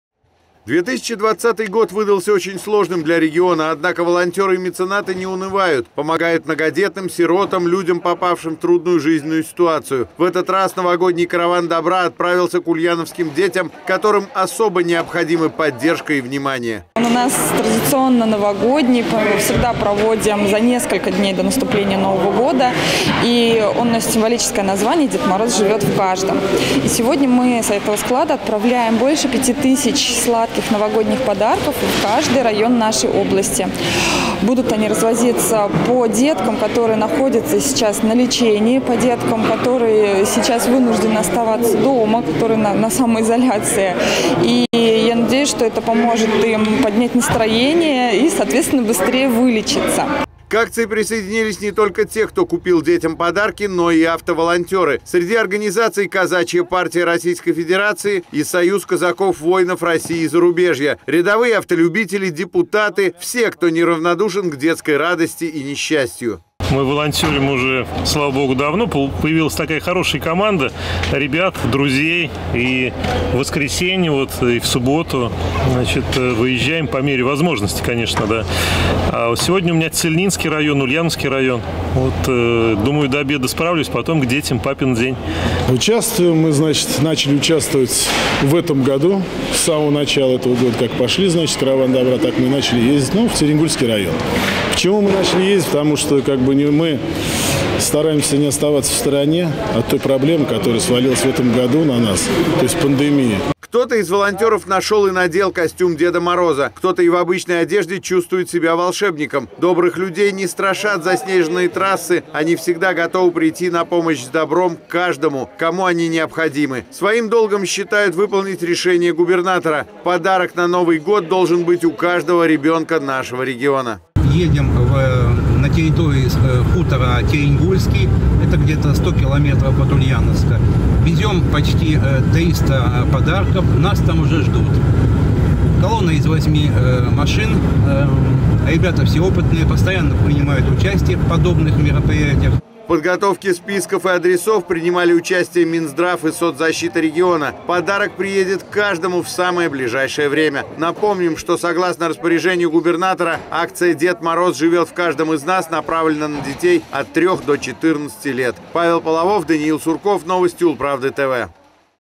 Новогодний «Караван добра» привез подарки детям
«Караваны добра» в нашем регионе стали уже доброй традицией. В этом году «Караван добра» объединился с акцией, запущенной губернатором Ульяновской области Сергеем Морозовым, «Дед Мороз живет в каждом из нас».